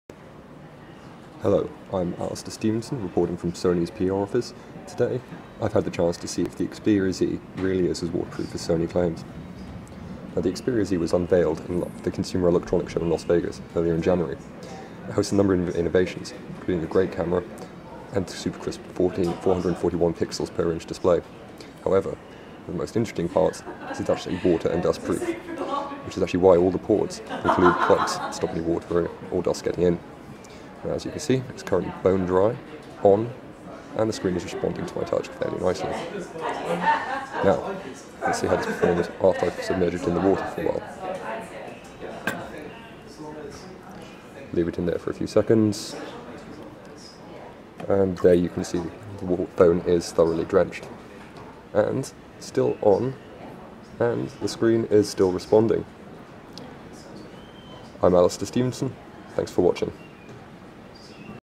Sony Xperia Z waterproof test
JAPANESE PHONE MAKER Sony unveiled the Xperia Z at CES this year, an Android phone that offers protection against the elements.

We put Sony's claim to the test, and visited the firm's London office to see if the phone really is as lifeproof as the company claims.